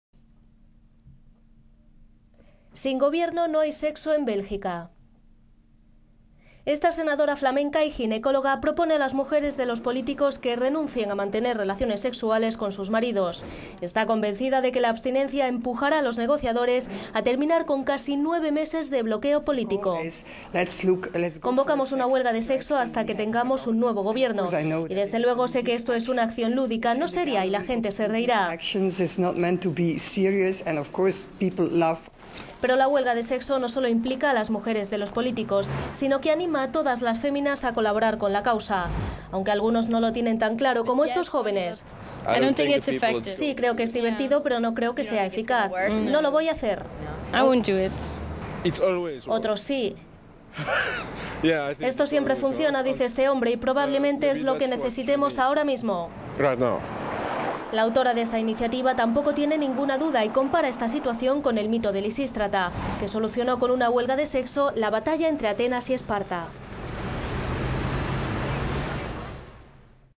Bélgica: sin gobierno no hay sexo
La senadora flamaneca y ginecóloga, Marleen Temmerman propone a las mujeres de los políticos que renuncien a mantener relaciones sexuales con sus maridos. Está convencida de que la abstiencia empujará a los negociadores a terminar con casi nueve meses de bloqueo político: "Convocamos una huelga de sexo hasta que tengamos un nuevo Gobierno. Y desde luego se que esto es una acción lúdica, no seria y la gente se reirá".